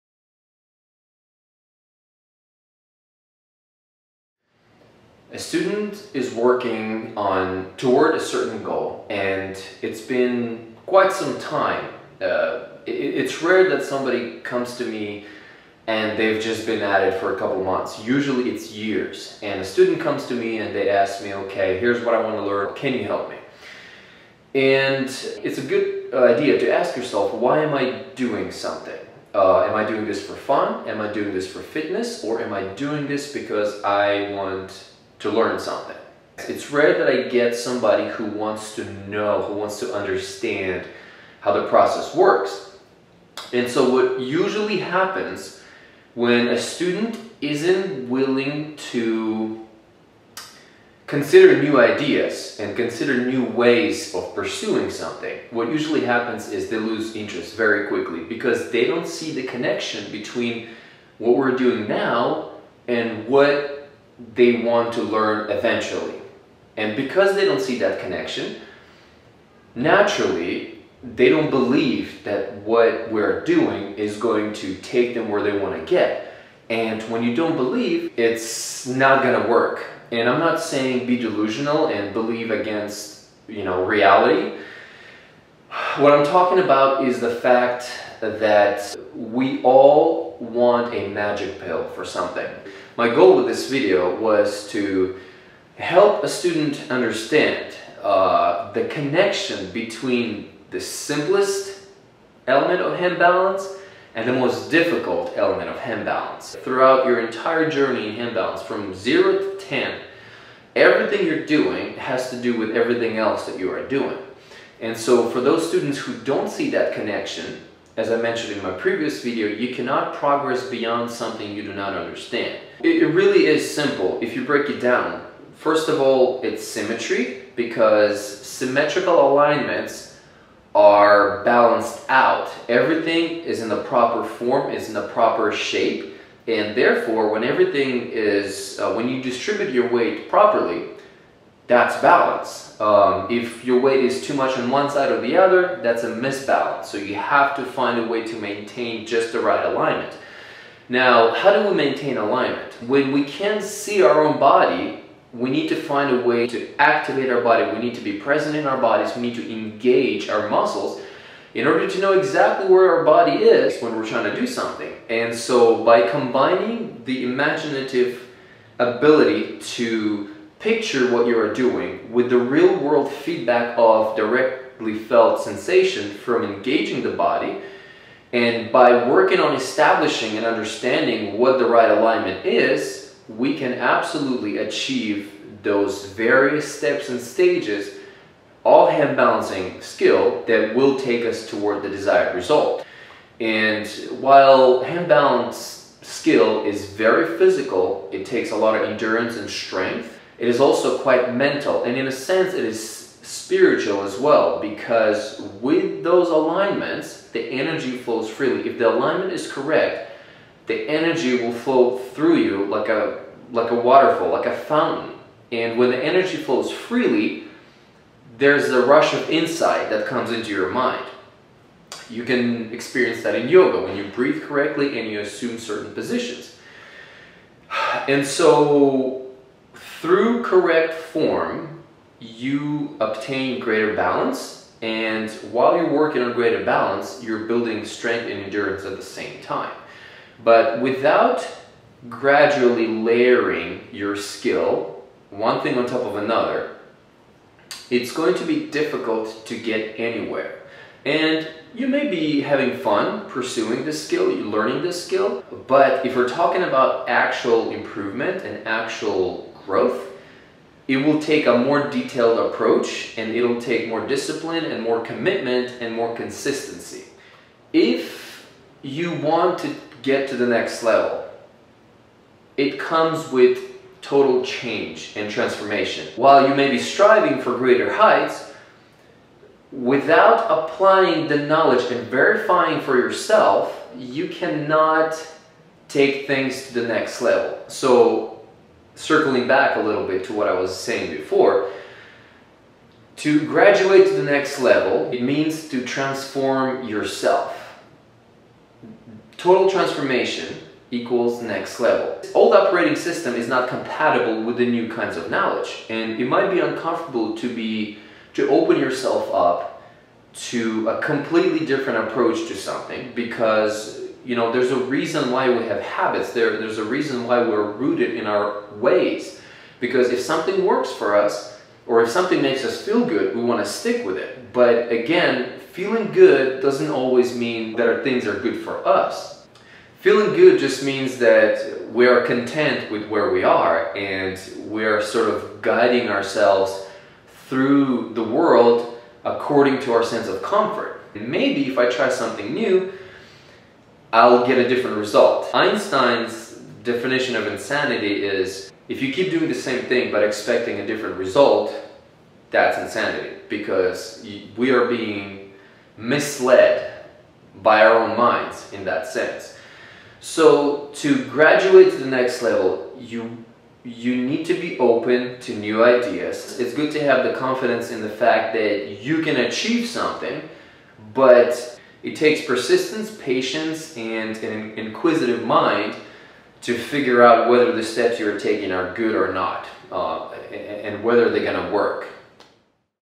Better Than "Magic Pill": Correct Strategy for Handstand Training and Progress
Many people start training handstands without clear definitions of intent. Bringing clarity of vision and direction to one's effort makes the ultimate difference.